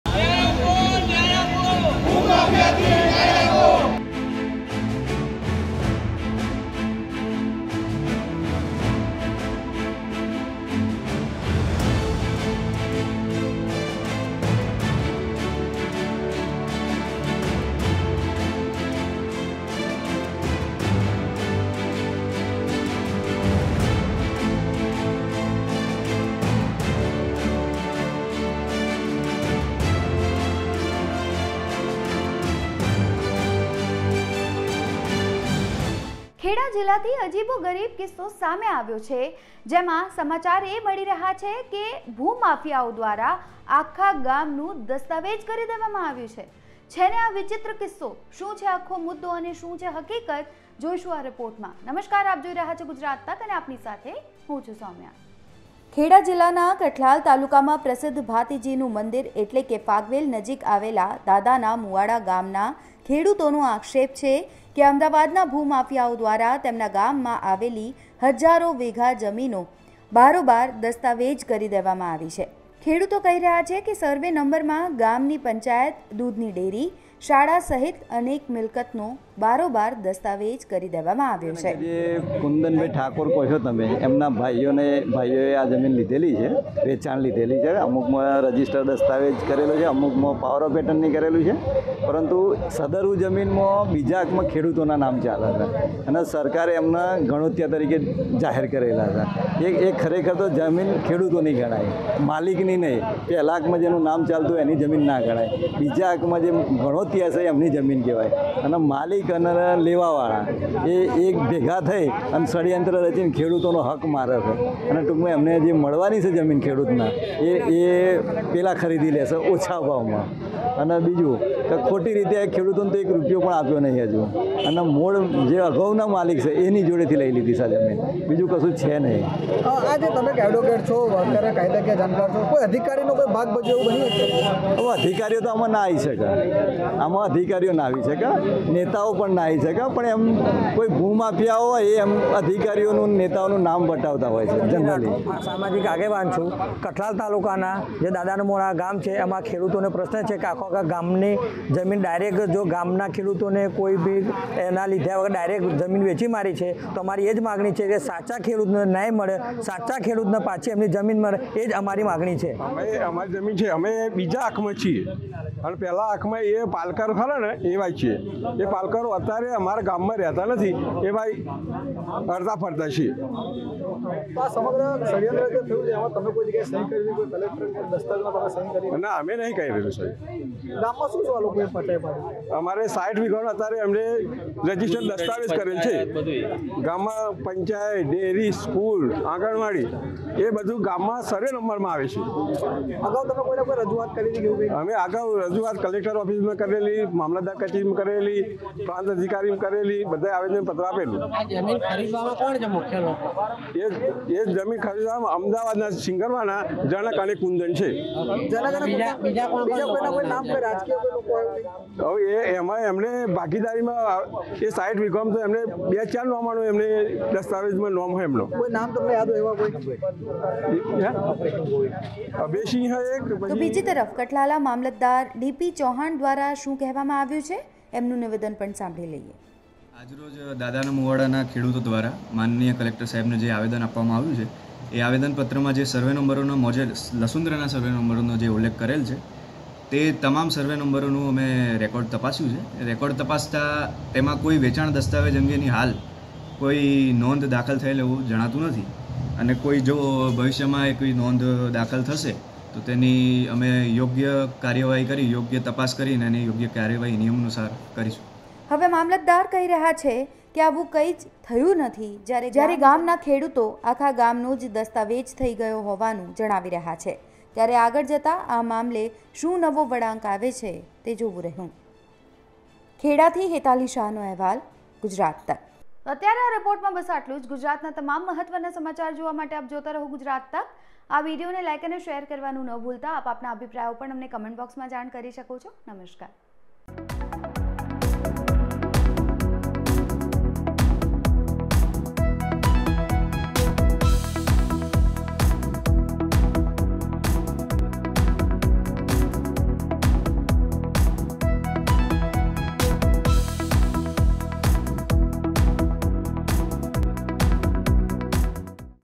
Kheda : ભૂમાફિયાઓએ આખા ગામનું કરી નાખ્યું? Gujarat Tak
Kheda: લ્યો બોલો… ભૂમાફિયાઓએ આખા ગામનો દસ્તાવેજ કરી નાખ્યો? જાણો શું છે મામલો
જિલ્લાના કઠલાલ તાલુકાના દાદાના મુવાડા ગામેથી અજીબો ગરીબ કિસ્સો સામે આવ્યો છે. જેમાં ગામના ખેડૂતોનો આક્ષેપ છે કે, ભૂમાફિયાઓ દ્વારા આખા ગામનો દસ્તાવેજ કરી દેવામાં આવ્યો છે.